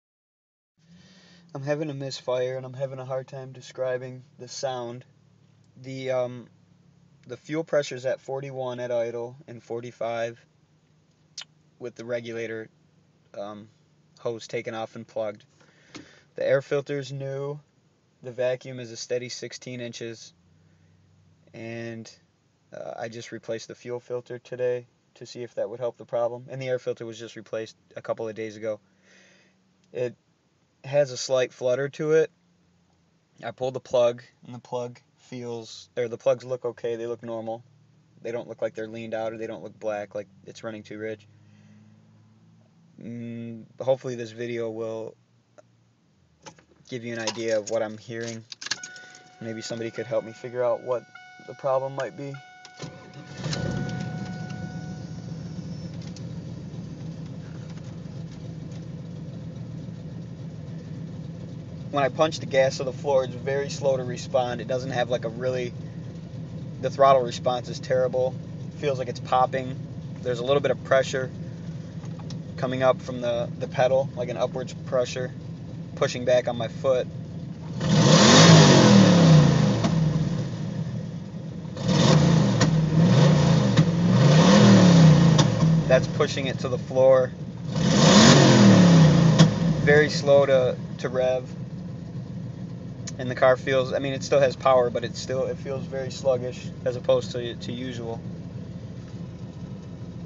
Cammed LT1 is misfiring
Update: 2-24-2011
So after replacing the fuel filter, air filter, and ruling out low fuel pressure and vacuum leaks through diagnostics, I decided to also rule out a clogged exhaust, so I pulled the Y pipe and ran open headers.  This problem still persisted, so I know it was something under the hood.

I decided to clean the throttle body and mass air flow sensor (I just got some cheap store brand throttle body cleaner, and some CRC Maf sensor cleaner).  I gave them both a thorough cleaning and got all the gum out of the throttle body, and all the dirt and grime off the MAF sensor.

This still didn't fix the problem, but the car did run better.  I pulled the battery from my car to put it in another car and an hour later put the battery back into my car.  Started it up, and wouldn't you know, the car ran like a top.  It's strange because after pulling the MAF sensor, I did pull the PCM BAT fuse for a minute to reset the pcm, but it did not do the trick.  It must have needed a good reset after fiddling with the MAF.

My guess is that the MAF was filthy from running with a dirty air filter for so long, and the cleaning worked wonders, and it just needed to be reset after being unplugged.  Maybe, maybe not, but once I had that battery out the car turned into a whole other animal.

Original Description:
For some reason the car has started to misfire the last couple days.  I am having a hard time tracking down exactly where the problem is.  I am ruling out the fuel system at this point.

I mention in the video that the car has a steady 16".  I know that 20-22" is normal for the LT1 but this car is cammed and since putting the cam in it has always held 16" of vacuum, so I am ruling out a vacuum leak as well.